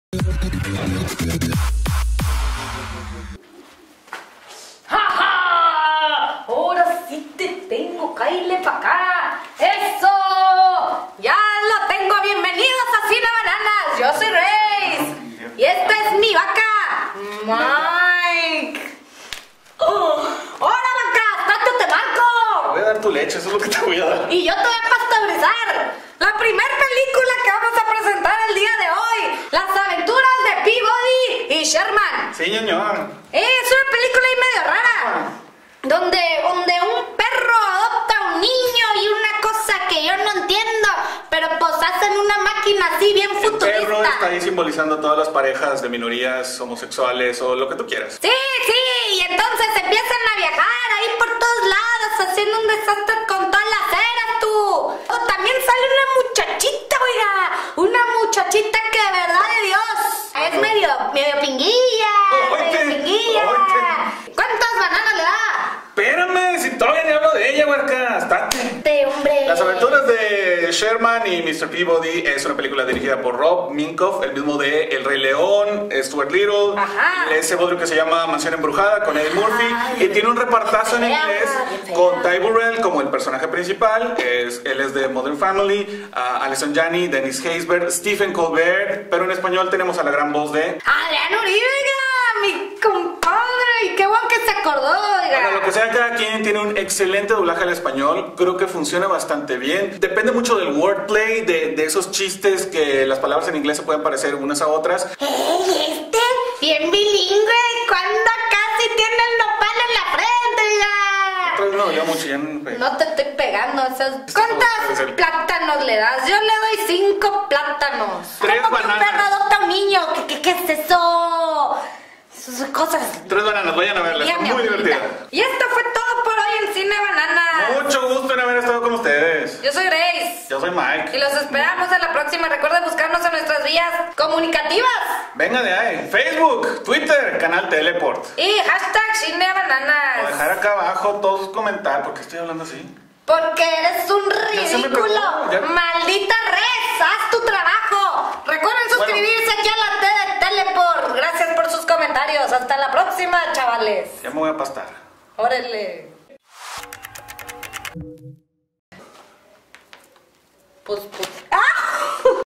CineBananas - Reseña de Las Aventuras de Sherman y Peabody (Mr. Peabody & Sherman)
CineBananas - Ep 17

Reseñas y recomendaciones cinematográficas con Grace y Mike. Estreno en salas: LAS AVENTURAS DE SHERMAN Y PEABODY (MR. PEABODY AND SHERMAN).